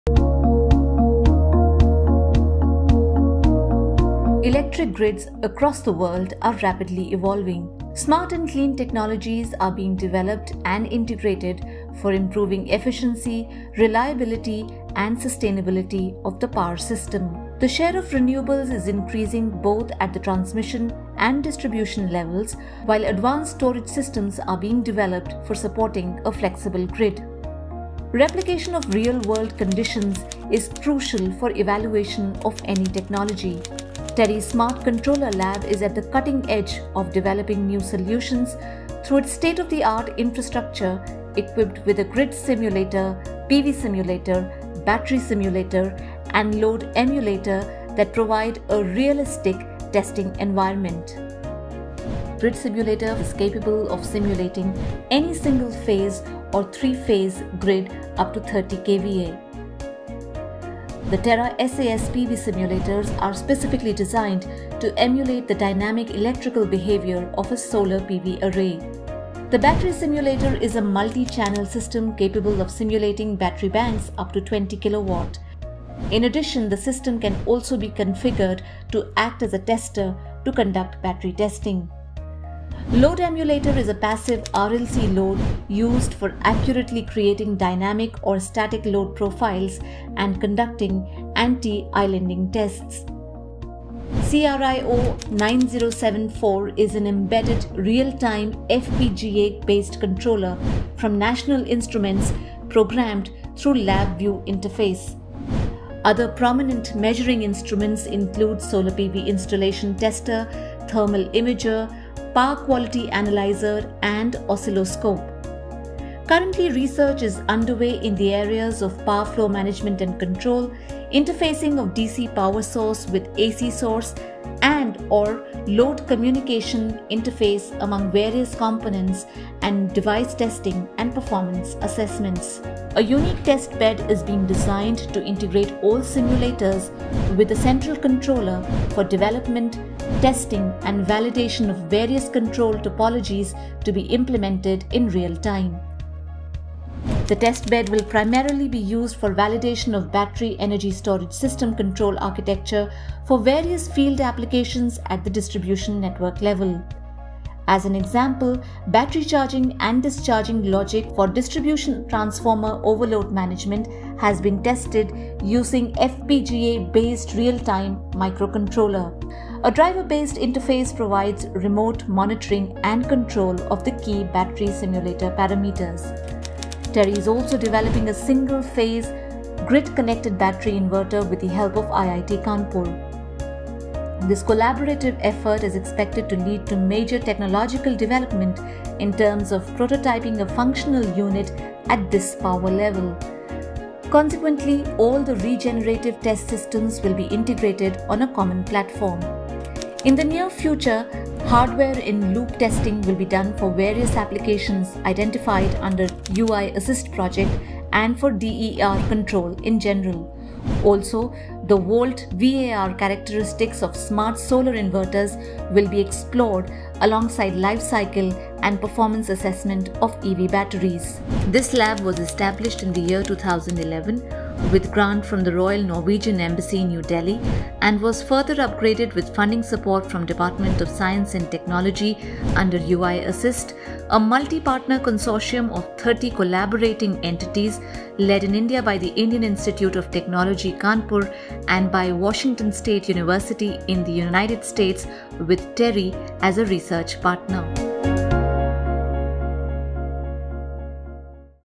Salient Features of TERI’s Smart Controller Laboratory
The Smart-Controller Laboratory at TERI was established with support from the Norwegian Embassy and was recently upgraded under the Indo-US project UI-ASSIST.
The state-of the-art infrastructure includes advanced power system simulators, emulators, and real-time embedded controllers. Power system control design and validation including control of Distributed Energy Resources and device testing and performance assessment are among the research focus.